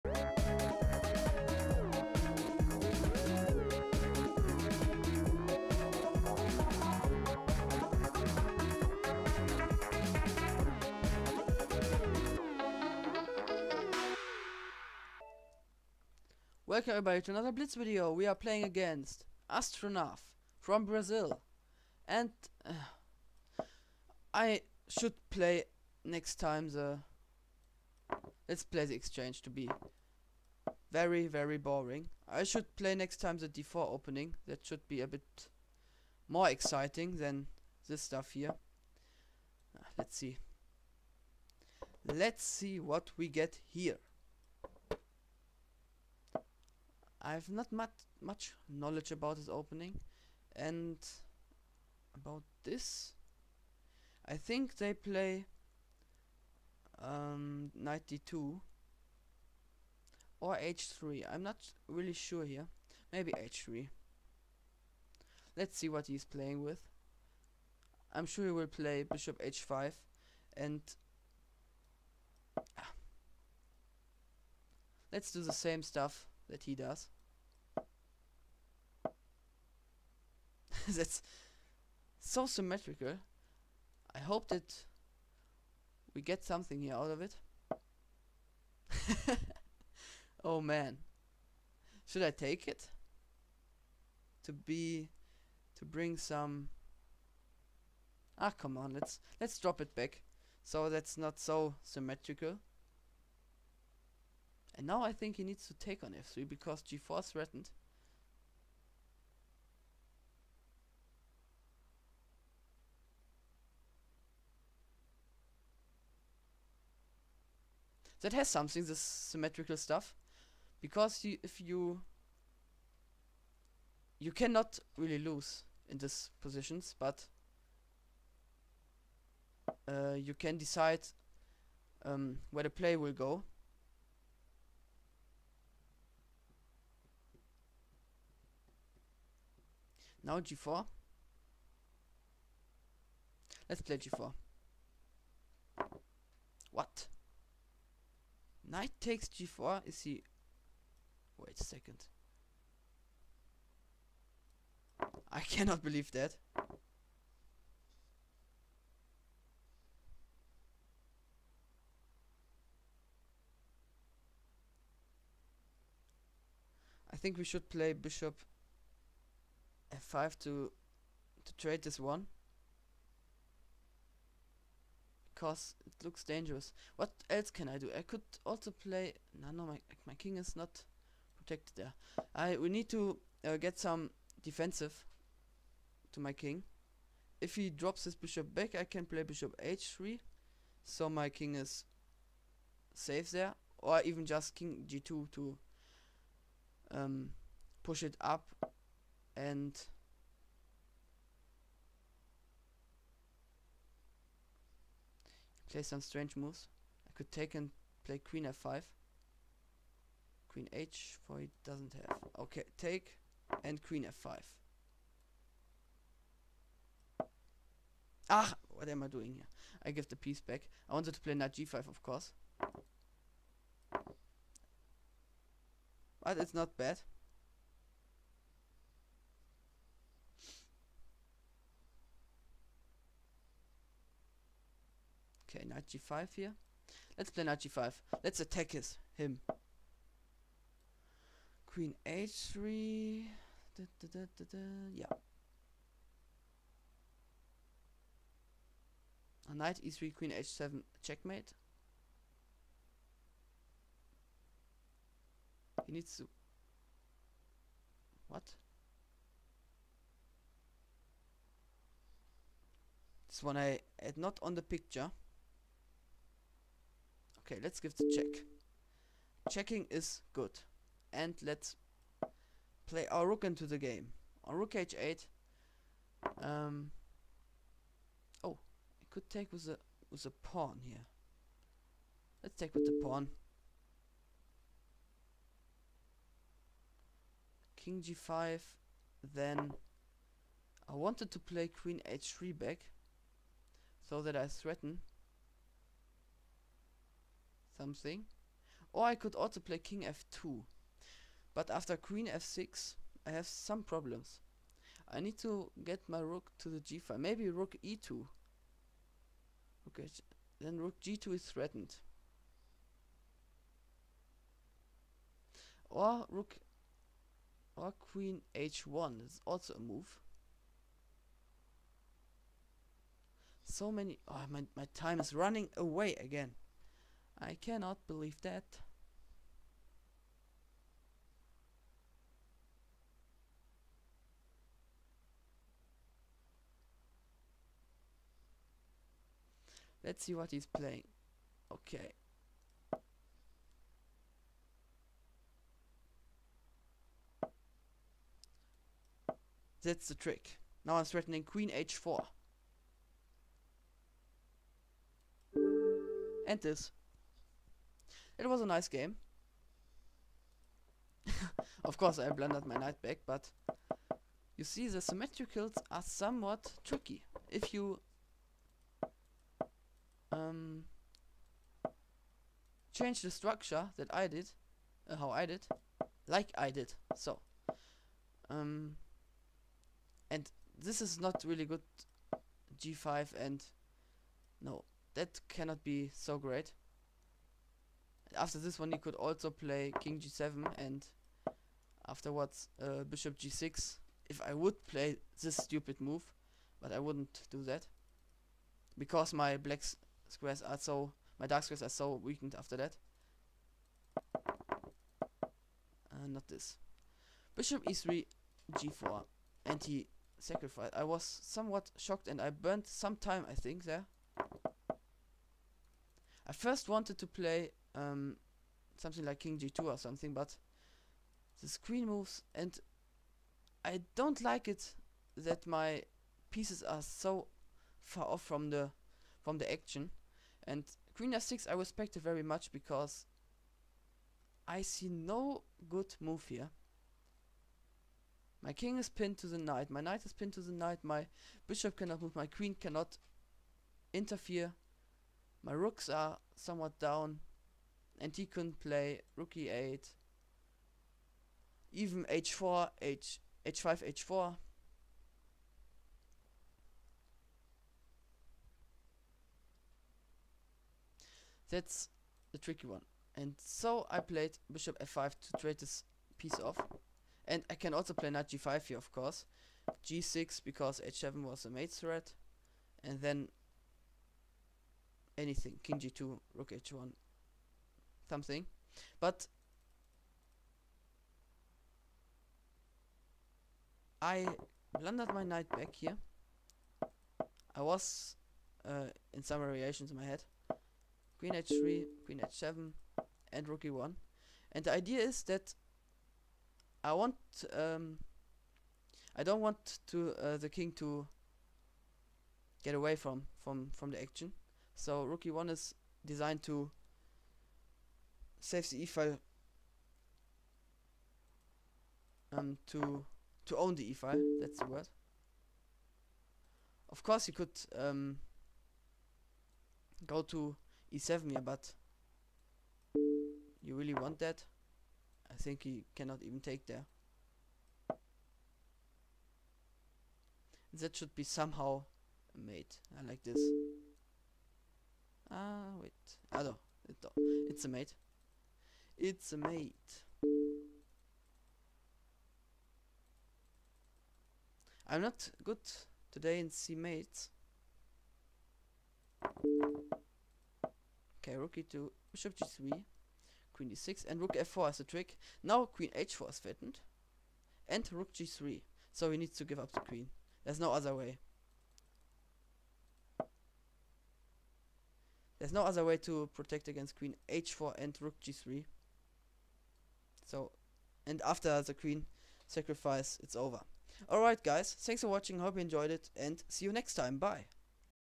LP Chess 5min #54 vs astronaf [1898] French
In this video we play against astronaf in a French defense. It is in an interesting game and we have a sub optimal start, but then this game delevopes to an interesting one. Have fun :)
Syromer B und auch STKMarvin freuen sich darauf :)